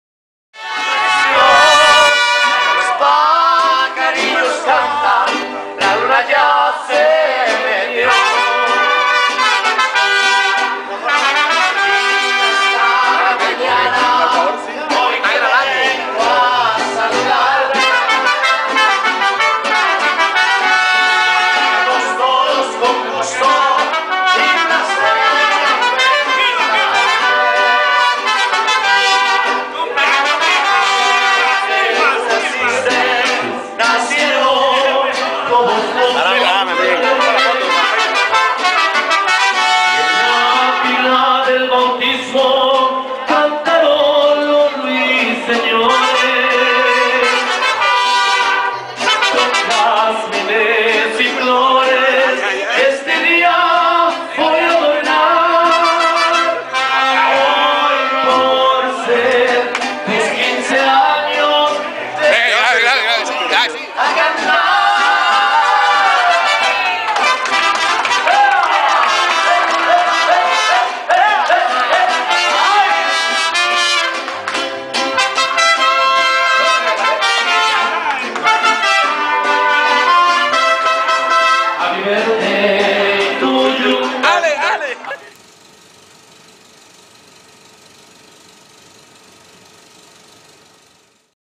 Festejando Con Mariachis, Diomedes Diaz
Diomedes Diaz: Celebrando El Cumpleaños En Bogota, 26 De Mayo. Material totalmente exclusivo de la página oficial del cacique en facebook.